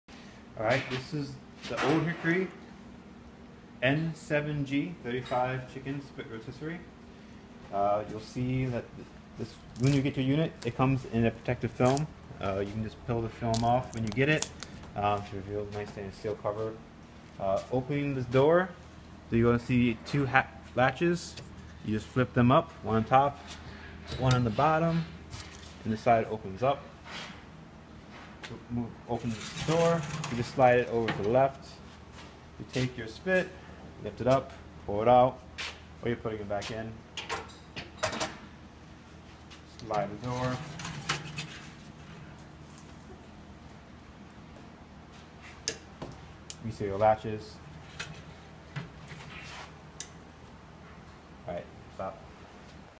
A Look Inside the Commercial Chicken Rotisserie Oven By Old Hickory, Model N7G 35
Find a wide selection of electric and gas chicken rotisseries for your restaurant from top brands like Old Hickory at Kitchenall.com

Kitchenall is your trusted supplier for commercial kitchen equipment. All units ship out within 24 hours from our New York location.